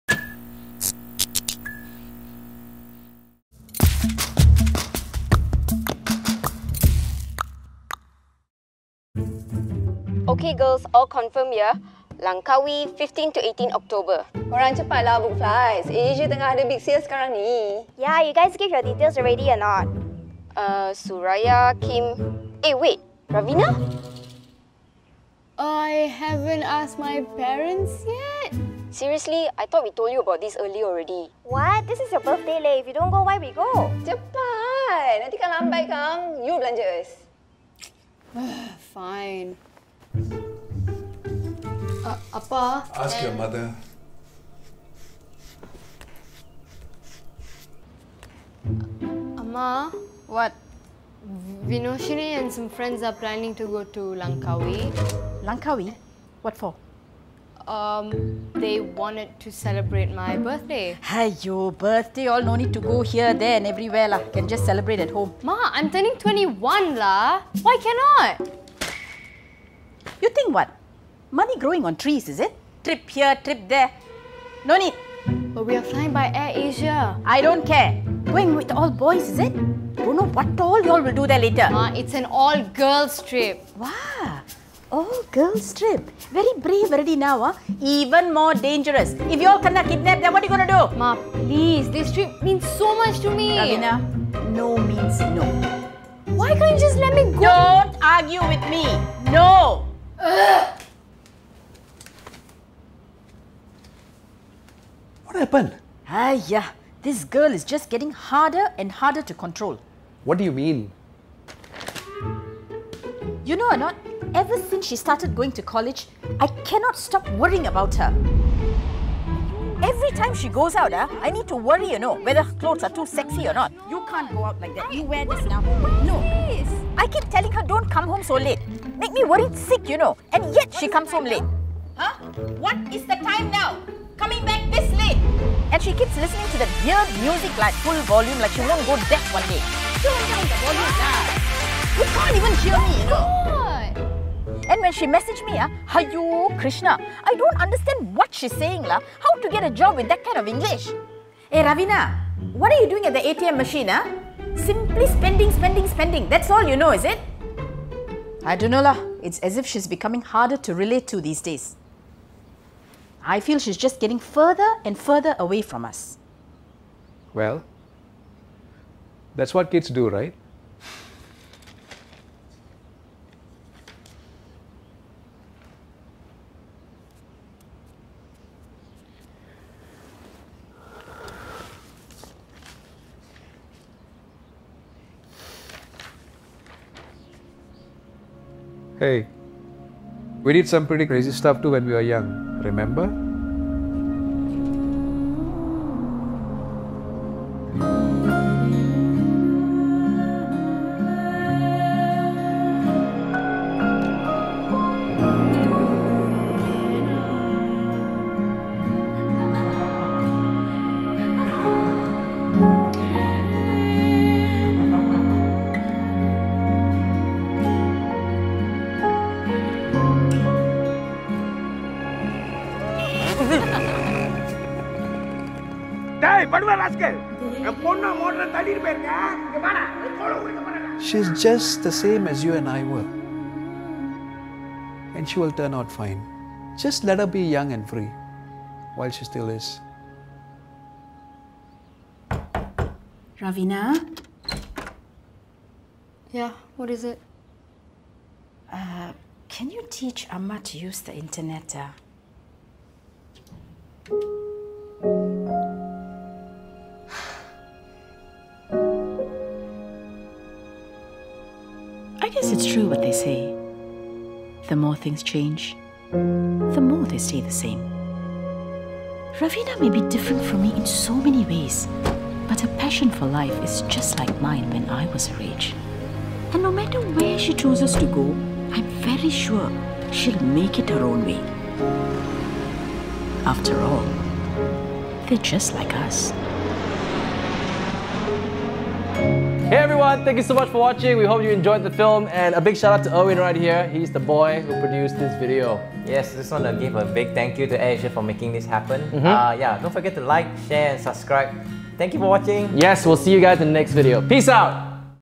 Just Like Us - Typical Asian Parents
Based on a true story, Raveena's friends were planning a trip to Langkawi to celebrate her birthday, but her over protective mom had to come in between their plans.

Written By Daniel Mok

Directed by:
Quan Wong

Production:
Jinnyboy Productions Sdn Bhd

Starring:
Sharmini Ramesh